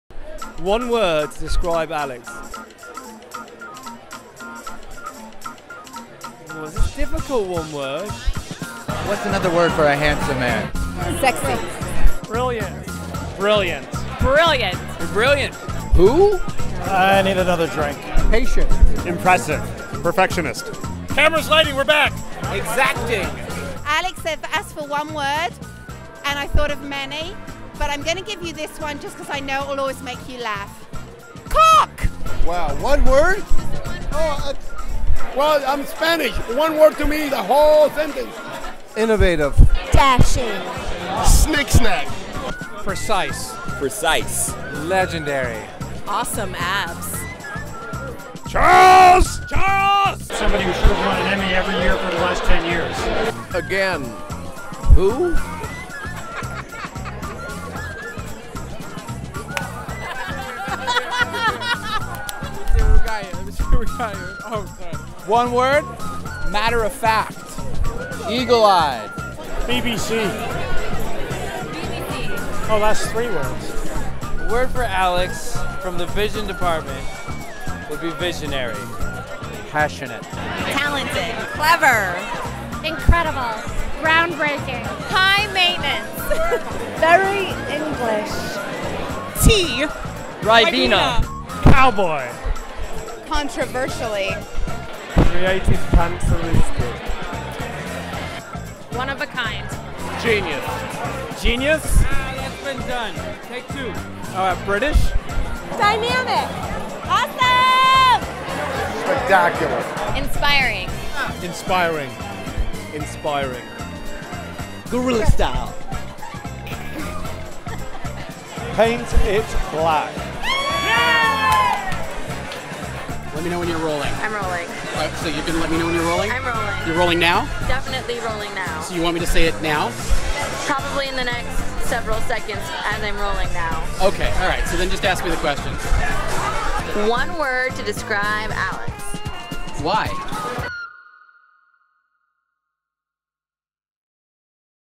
Alex Rudzinski - One Word Interviews
One word interviews for Alex from his DWTS family